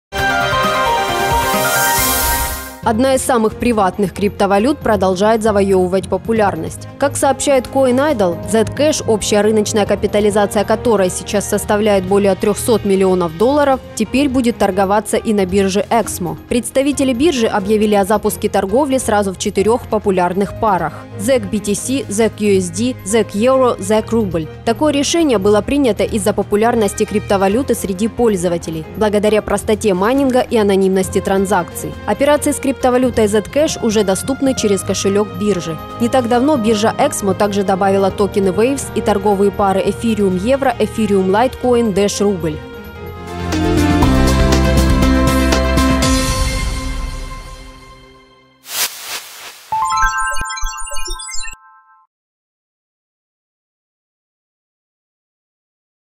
KCN Zcash теперь доступна на EXMO
Одна из самым приватных криптовалют продолжает завоевывать популярность. Как сообщает Coinidol, Zcash, общая рыночная капитализация которой сейчас составляет более $300 млн,  теперь будет торговаться и на бирже EXMO. Представители биржи объявили о запуске торговли сразу в четырех популярных парах: ZEC/BTC, ZEC/USD, ZEC/EUR, ZEC/RUB. Такое решение было принято из-за популярности криптовалюты среди пользователей благодаря простоте майнинга и анонимности транзакций. Операции с криптовалютой Zcash уже доступны через кошелек биржи. Не так давно биржа EXMO также добавила токены Waves и торговые пары ETH/EUR, ETH/LTC, DASH/RUB.